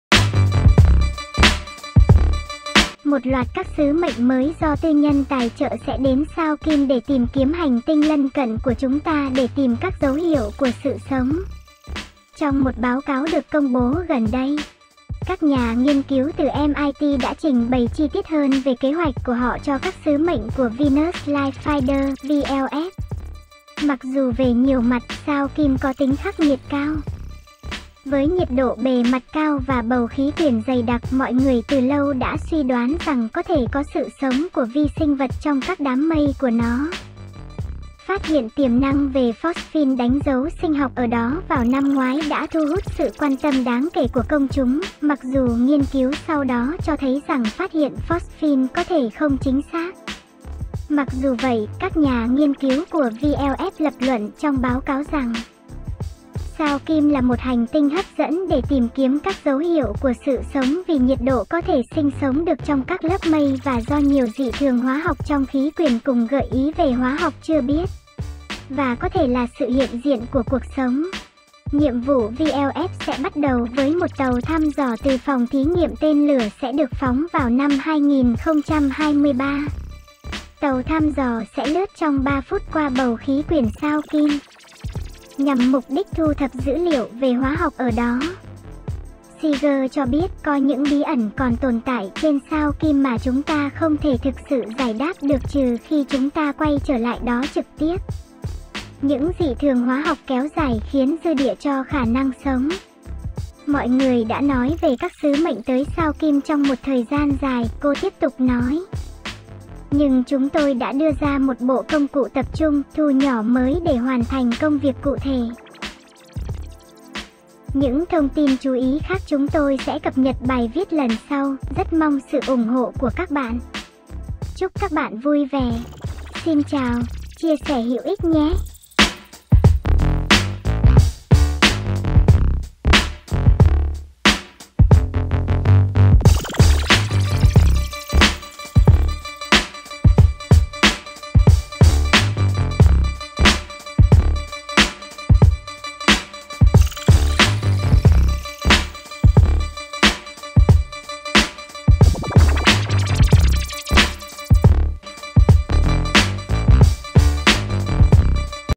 Sự sống trên sao Kim các nhà nghiên cứu của MIT lên kế hoạch tìm kiếm
Một loạt các sứ mệnh mới do tư nhân tài trợ sẽ đến Sao Kim để tìm kiếm hành tinh lân cận của chúng ta để tìm các dấu hiệu của sự sống. Trong một báo cáo được công bố gần đây, các nhà nghiên cứu từ MIT đã trình bày chi tiết hơn về kế hoạch của họ cho các sứ mệnh của Venus Life Finder (VLF).

Mặc dù về nhiều mặt, sao Kim có tính khắc nghiệt cao, với nhiệt độ bề mặt cao và bầu khí quyển dày đặc, mọi người từ lâu đã suy đoán rằng có thể có sự sống của vi sinh vật trong các đám mây của nó. Phát hiện tiềm năng về phosphine đánh dấu sinh học ở đó vào năm ngoái đã thu hút sự quan tâm đáng kể của công chúng, mặc dù nghiên cứu sau đó cho thấy rằng phát hiện phosphine có thể không chính xác .

Mặc dù vậy, các nhà nghiên cứu của VLF lập luận trong báo cáo rằng, “Sao Kim là một hành tinh hấp dẫn để tìm kiếm các dấu hiệu của sự sống vì nhiệt độ có thể sinh sống được trong các lớp mây và do nhiều dị thường hóa học trong khí quyển cùng gợi ý về hóa học chưa biết và có thể là sự hiện diện của cuộc sống. ”

Nhiệm vụ VLF sẽ bắt đầu với một tàu thăm dò từ Phòng thí nghiệm Tên lửa sẽ được phóng vào năm 2023. Tàu thăm dò sẽ lướt trong ba phút qua bầu khí quyển Sao Kim, nhằm mục đích thu thập dữ liệu về hóa học ở đó.

Seager cho biết: “Có những bí ẩn còn tồn tại trên sao Kim mà chúng ta không thể thực sự giải đáp được trừ khi chúng ta quay trở lại đó trực tiếp. "Những dị thường hóa học kéo dài khiến dư địa cho khả năng sống."

“Mọi người đã nói về các sứ mệnh tới Sao Kim trong một thời gian dài,” cô tiếp tục nói. “Nhưng chúng tôi đã đưa ra một bộ công cụ tập trung, thu nhỏ mới để hoàn thành công việc cụ thể.”